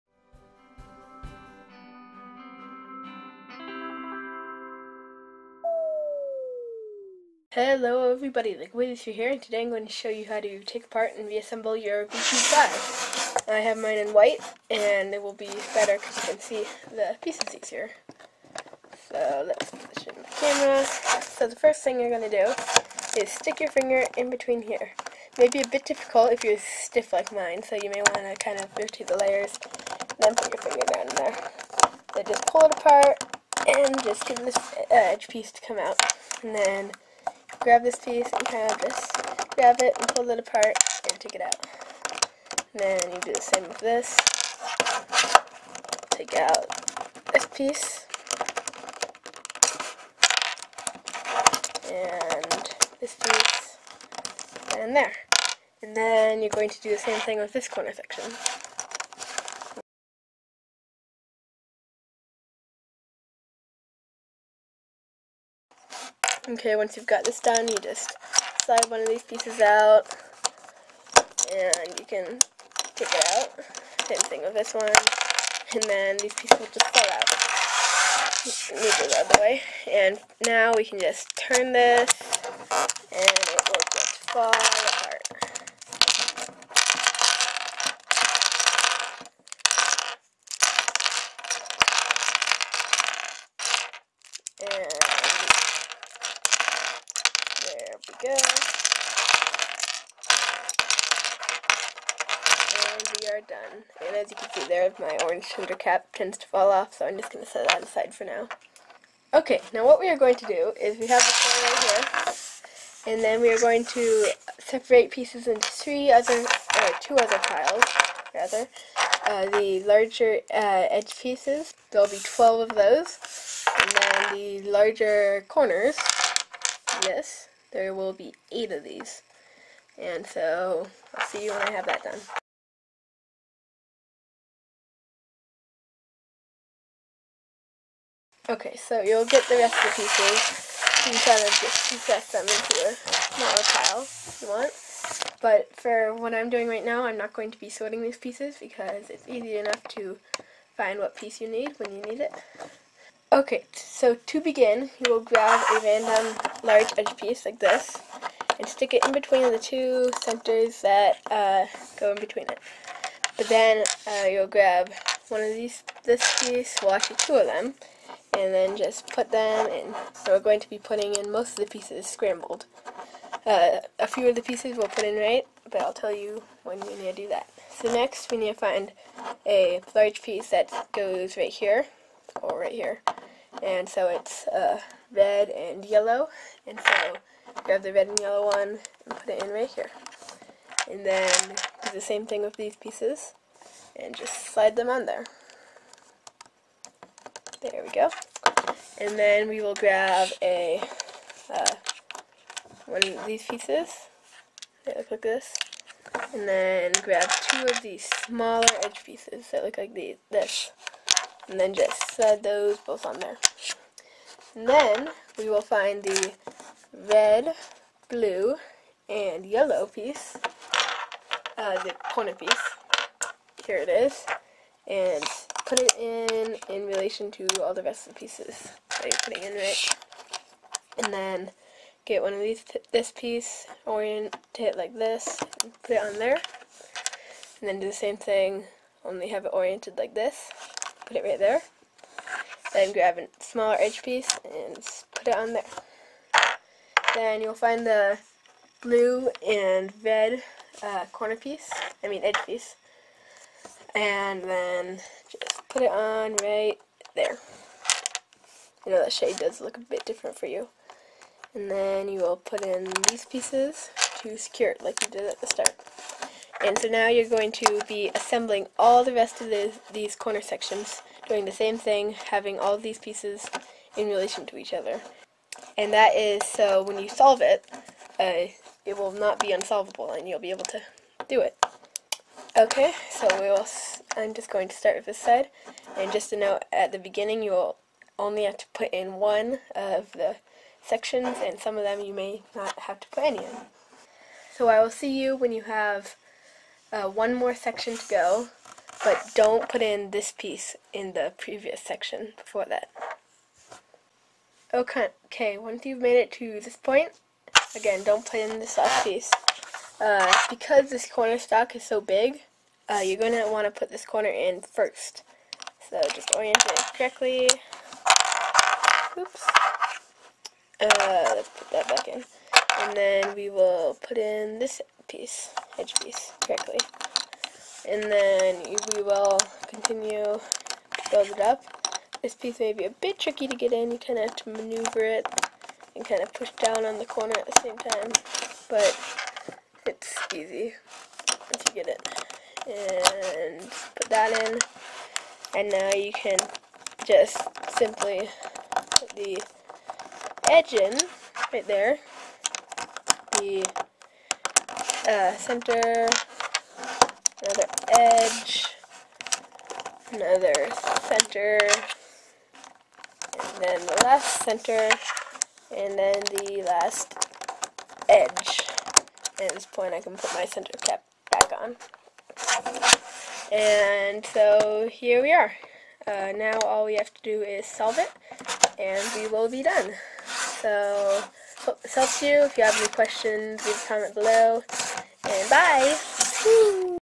V-Cube 5 Disassembly and Assembly Tutorial
How to take apart and reassemble a V-Cube 5.